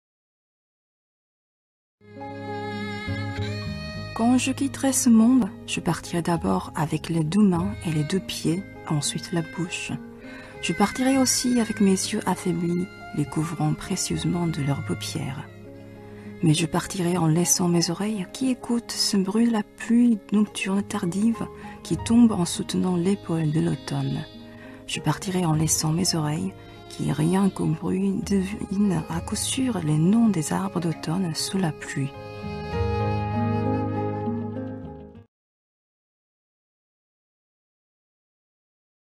Funérailles au vent 27, Tong-gyu Hwang
lecture par Yun Yennie
traduit du coréen par Hyunja Kim-Schmidt et  Thierry Gillybœuf

Funérailles au vent 27

Quand je quitterai ce monde
Je partirai d’abord avec les deux mains et les deux pieds, ensuite la bouche.
Je partirai aussi avec mes yeux affaiblis les couvrant précieusement de leurs paupières.
Mais je partirai en laissant mes oreilles qui écoutent
Ce bruit de la pluie nocturne tardive
Qui tombe en soutenant l’épaule de l’automne.
Je partirai en laissant mes oreilles
Qui rien qu’au bruit devinent à coup sûr les noms des arbres d’automne sous la pluie.